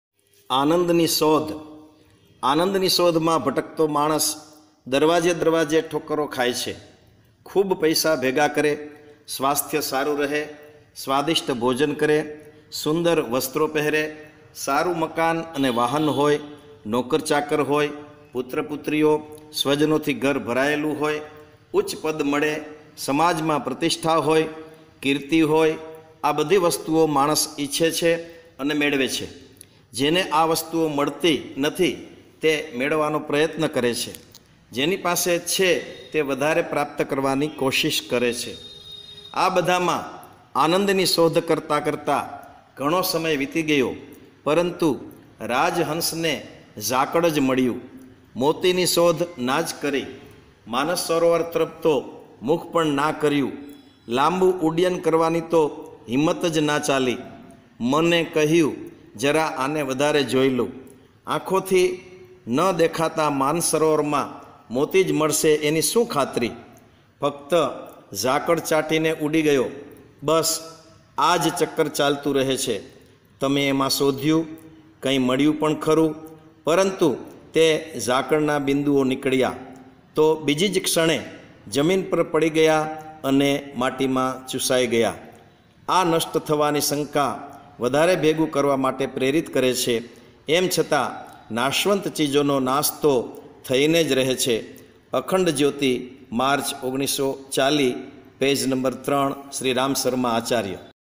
Aanand ni Shodh : આનંદની શોધ “યુગ ચેતના”
ઋષિચિંતનના સાંનિધ્યમાં વર્તમાન યુગની સમસ્યાઓ નું સમાધાન માટે, અને દુર્બુદ્ધિ ને દૂર કરી સદ્ બુદ્ધિ સ્થાપના માટેનો “ઋષિ ચિંતન”